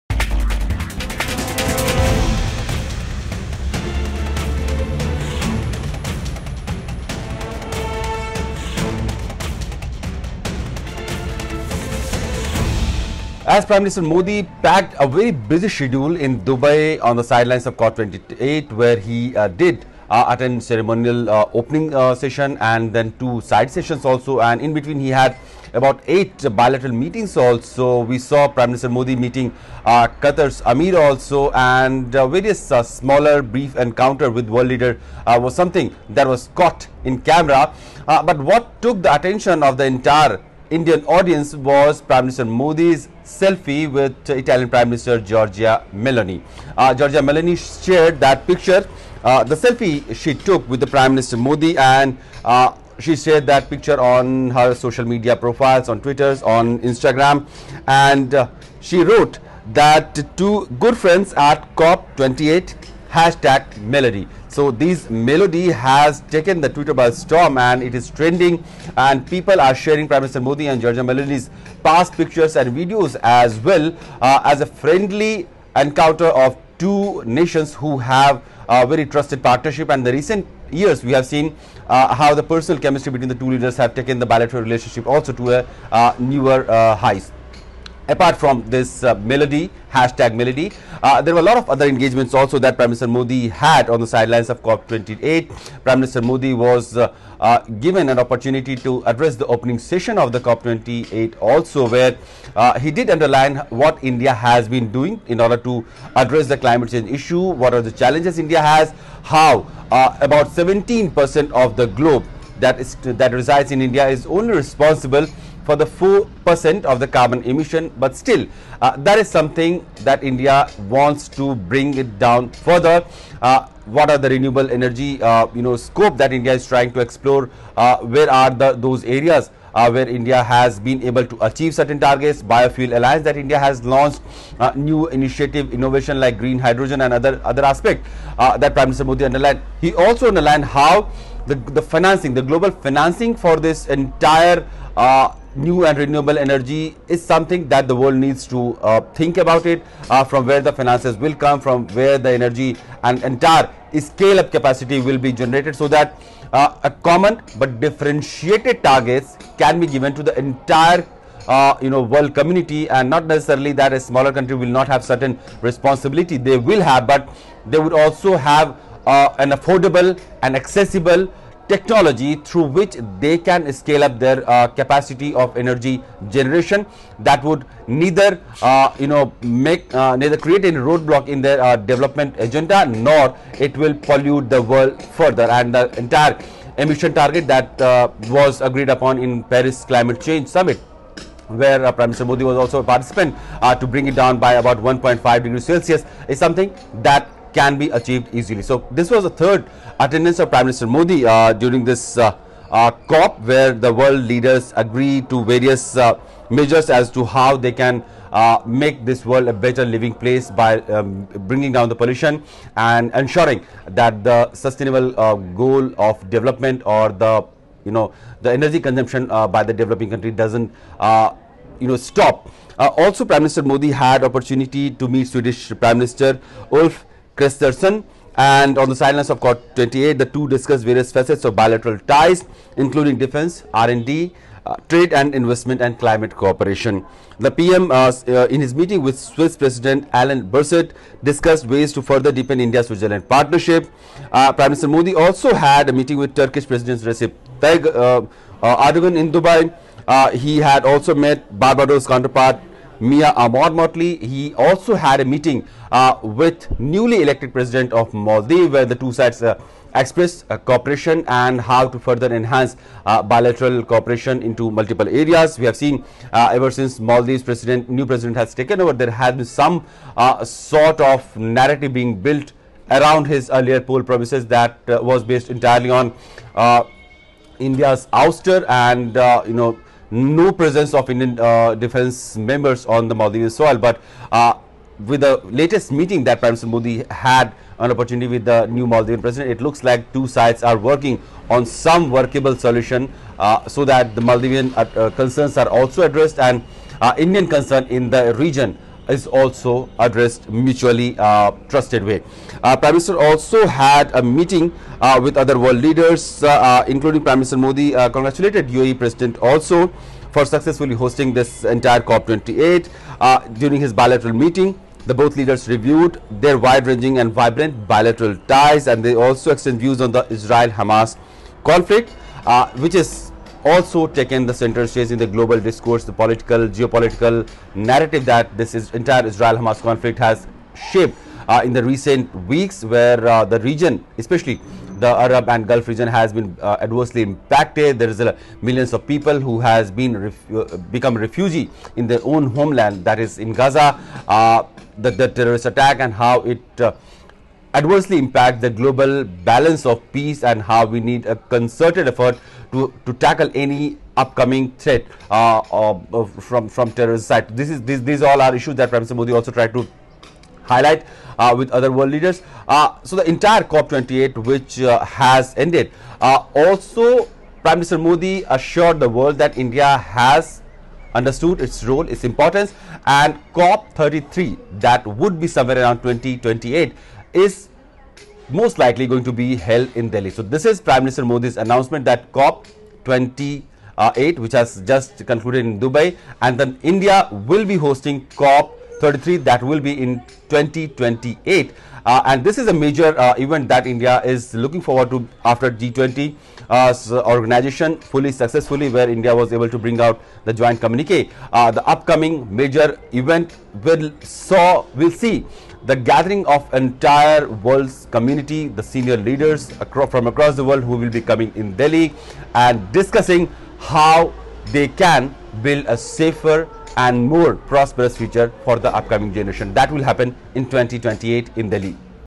PM Modi And Meloni | PM Modi News | PM Modi And Meloni In Dubai For COP28 | N18V | News18
Italian Prime Minister Giorgia Meloni posted a selfie with Prime Minister Narendra Modi taken during their meeting on sidelines of the ongoing 28th Conference of the Parties (COP28) in Dubai on Friday. Meloni clicked a selfie with the Indian prime minister and shared it on her Instagram account. “Good friends at COP28 Melodi,” Meloni captioned her selfie with PM Modi.